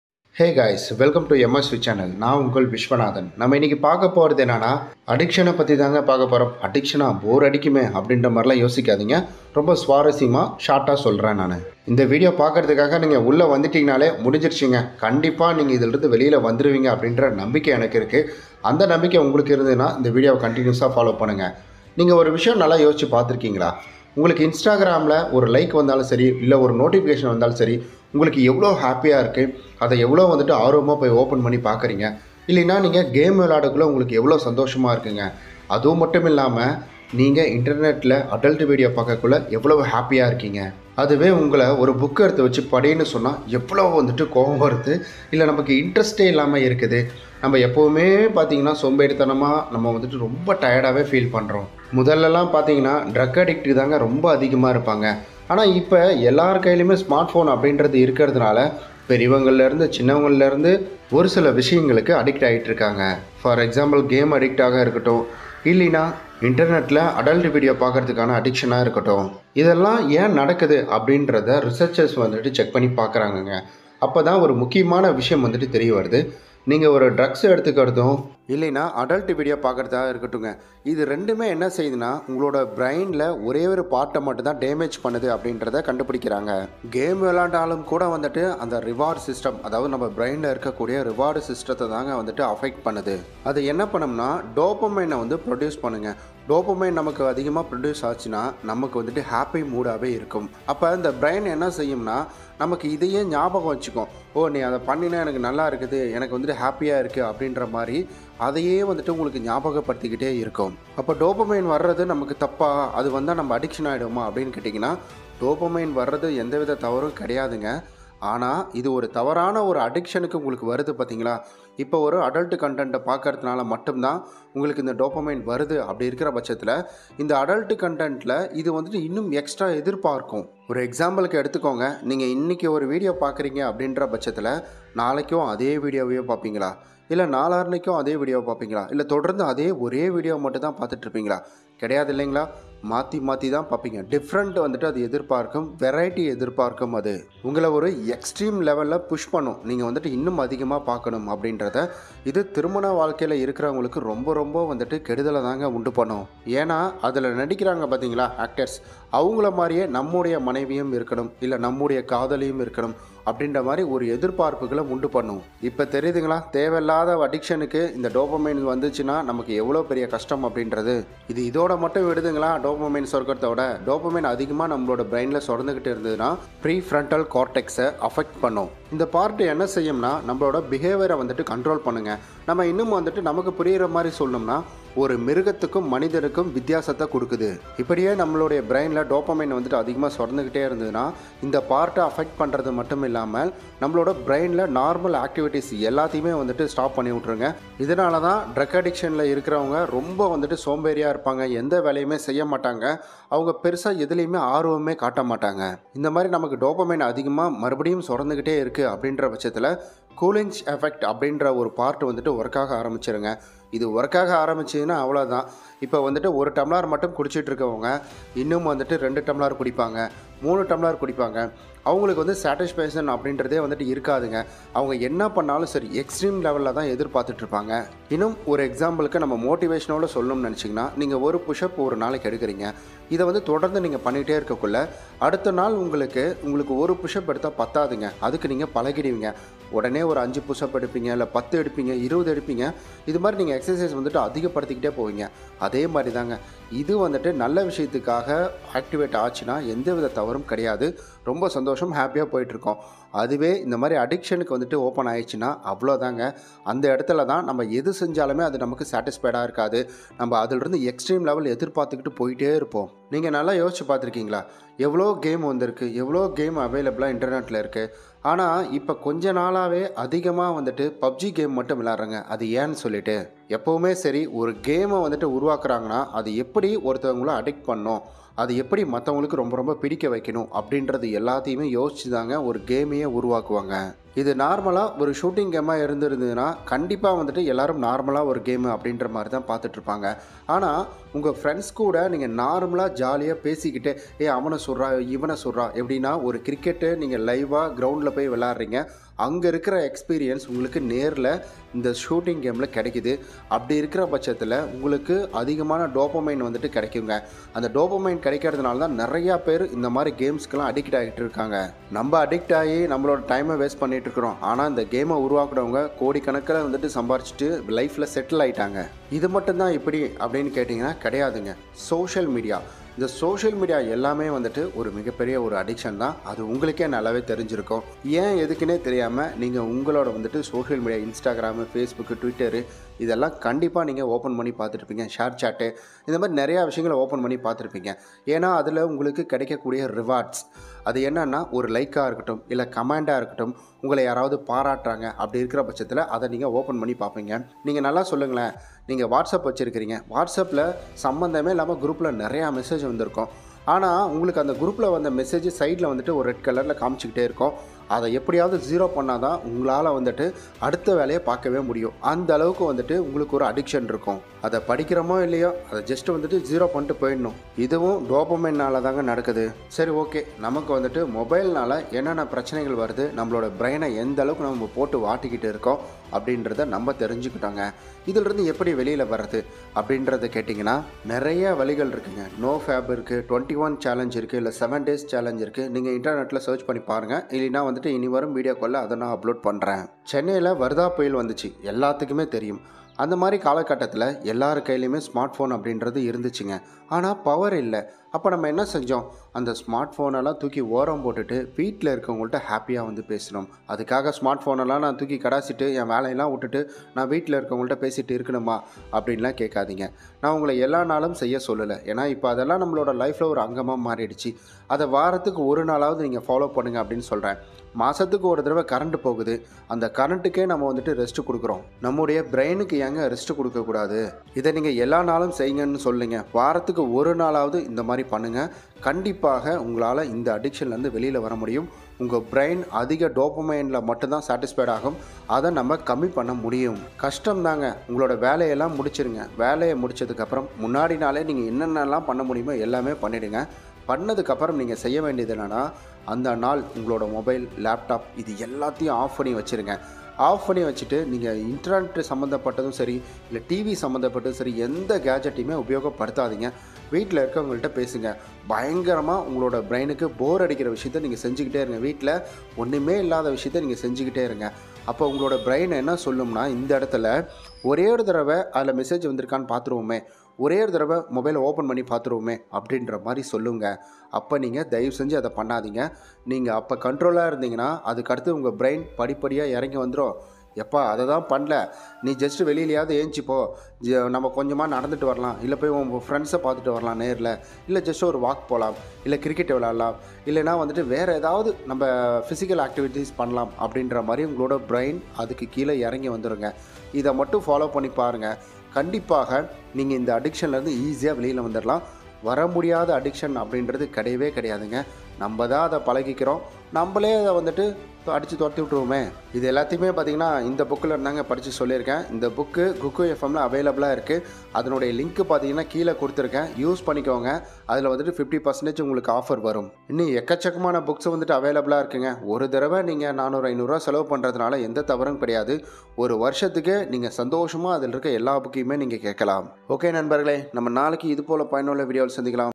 STOP THESE BRAIN DAMAGING HABITS |NOFAP/DOPAMINE DETOX/FAST TAMIL|Mobile addiction
DopamineDetox motivational Speech Tamil